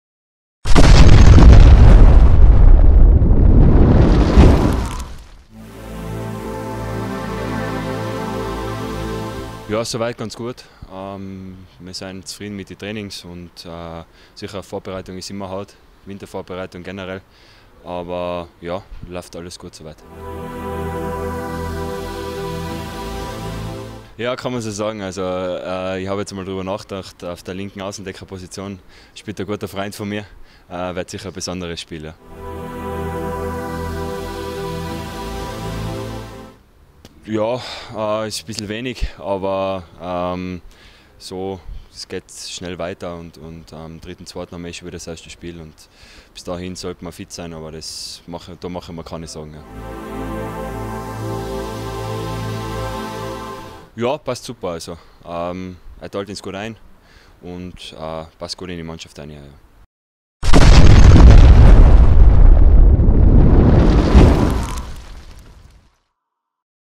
SCRA TV 2018 Wintervorbereitung - Christian Gebauer
Ex-Wattens Akteur Christian Gebauer freut sich auf das 1. Testspiel des Jahres, denn unsere Nummer 9 trifft mit dem SCRA auf seine ehemaligen Kollegen aus dem Tirol.